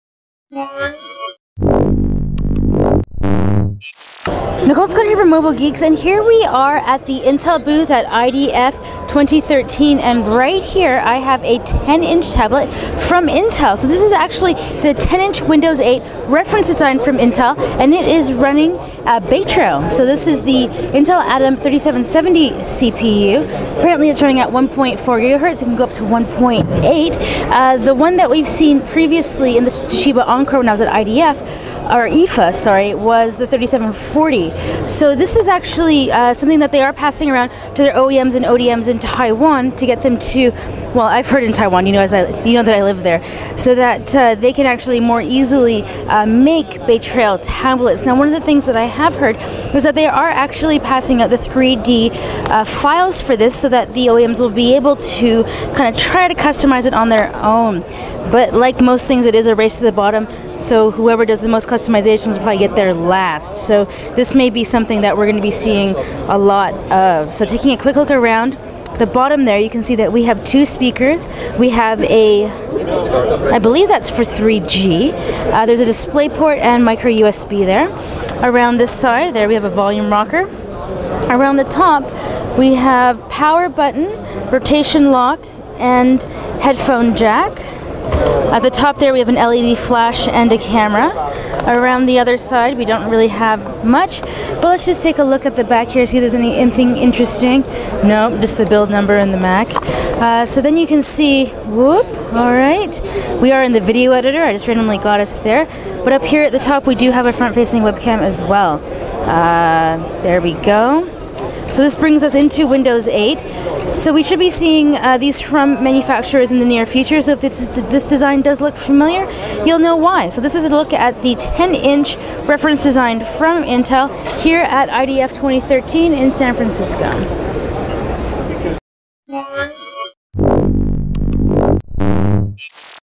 Intel Baytrail Windows 8 Reference Design - 10.1 inch HD Tablet
Baytrail reference design Intel is coming out swinging with BayTrail Tablets and they've got some reference designs that they've been passing out to their partners to help speed up the process of getting these beauties to market. We take a look at a 10.1 inch HD tablet running Windows 8.1 with a Atom Z3770 processor.